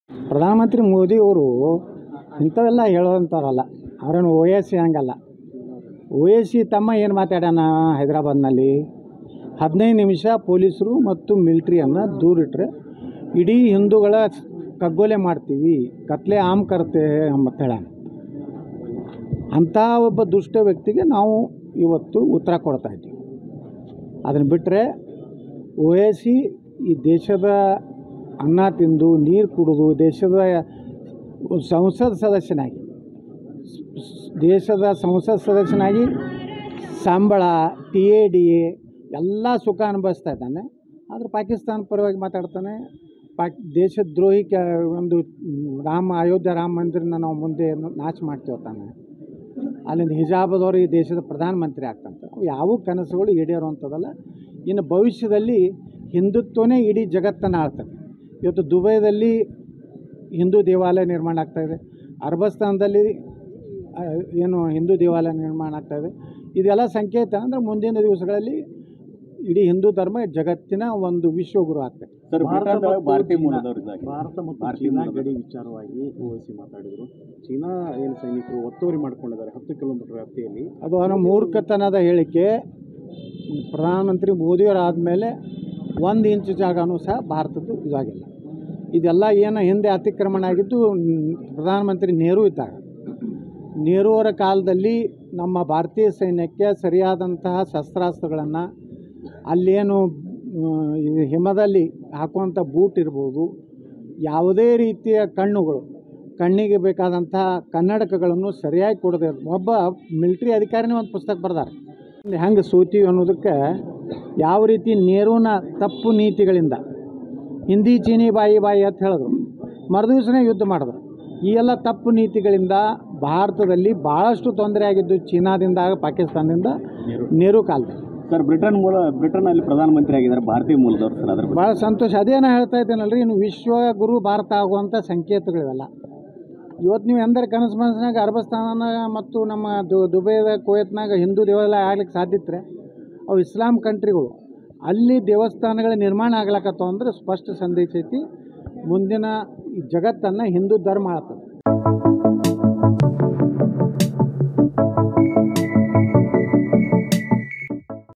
ಪಾಕಿಸ್ತಾನ, ಅಫ್ಘಾನಿಸ್ತಾನ ಭಾರತದ ಅವಿಭಾಜ್ಯ ಅಂಗವಾಗಬೇಕೆಂಬ ಕನಸು: ಶಾಸಕ ಯತ್ನಾಳ್
► "ನೆಹರೂ ದೇಶದ ಸೈನಿಕರಿಗೆ ಏನೂ ವ್ಯವಸ್ಥೆ ಮಾಡಿ ಕೊಟ್ಟಿರಲಿಲ್ಲ"

► "ಇಸ್ಲಾಮಿಕ್ ರಾಷ್ಟ್ರಗಳಲ್ಲೇ ದೇವಸ್ಥಾನಗಳು ನಿರ್ಮಾಣ ಆಗ್ತಾ ಇದೆ.."

► "ಉವೈಸಿ ದೇಶದ ಸಂಸದನಾಗಿ, ಪಾಕಿಸ್ತಾನದ ಪರ ಮಾತಾಡ್ತಾನೆ "